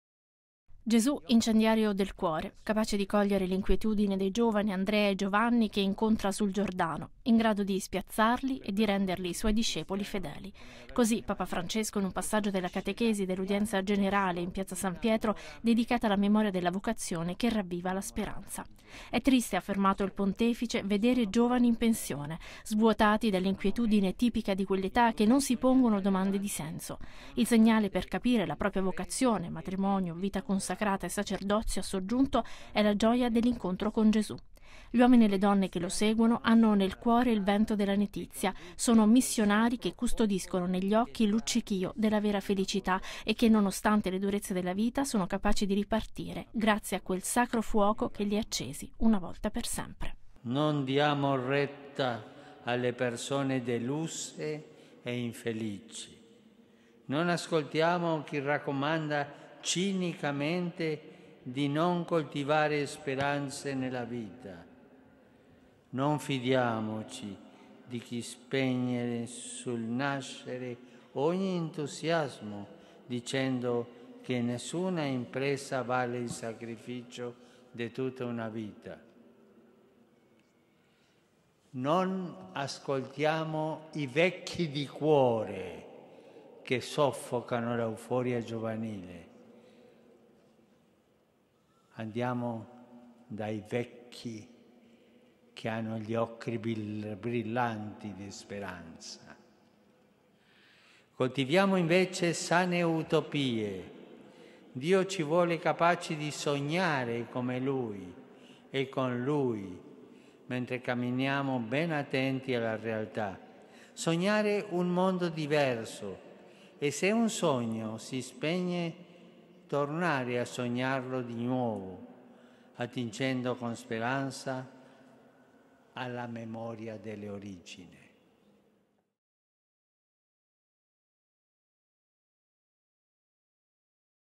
Udienza generale 30-08-2017
Non mandiamo in pensione la capacità di sognare

Papa Francesco, all’udienza generale in Piazza San Pietro, ha esortato a seguire Gesù avendo nel cuore il vento della letizia e custodendo negli occhi il luccichio della vera felicità